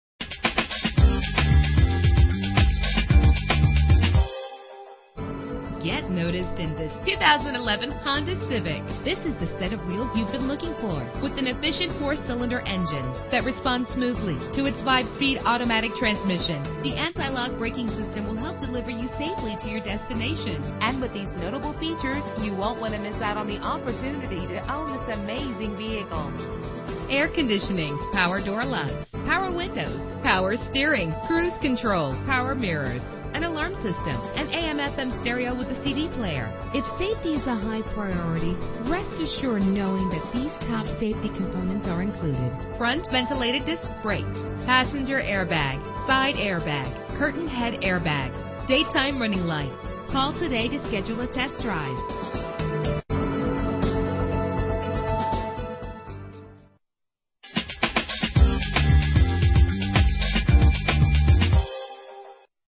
2011 Honda Civic - Vadnais MN
Year: 2011
Make: Honda
Model: Civic
Trim: LX
Engine: 1.8 liter inline 4 cylinder 16 valve
Transmission: 5-Speed Automatic
Color: Black
Mileage: 0
Address:
3360 Highway 61